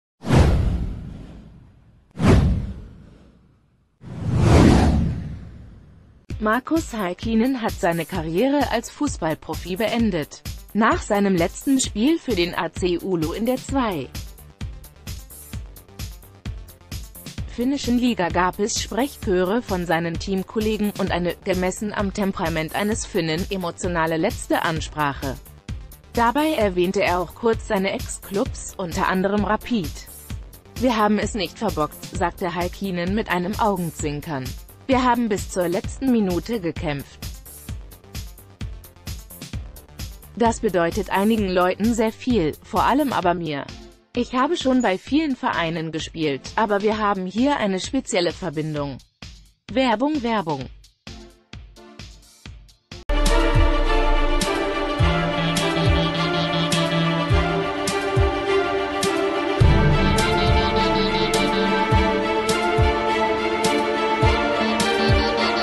Markus Heikkinens emotionale letzte Kabinen-Ansprache: Das bedeutet mir viel
Markus Heikkinen hat seine Karriere als Fußball-Profi beendet. Nach seinem letzten Spiel für den AC Oulu in der 2. finnischen Liga gab es Sprechchöre von seinen Teamkollegen, und eine - gemessen am Temprament eines Finnen - emotionale letzte Ansprache. Dabei erwähnte er auch kurz seine Ex-Klubs, unter anderem Rapid. "Wir haben es nicht verbockt", sagte Heikkinen mit einem Augenzwinkern. "Wir haben bis zur letzten Minute gekämpft. Das bedeutet einigen Leuten sehr viel - vor allem aber mir. Ich ha...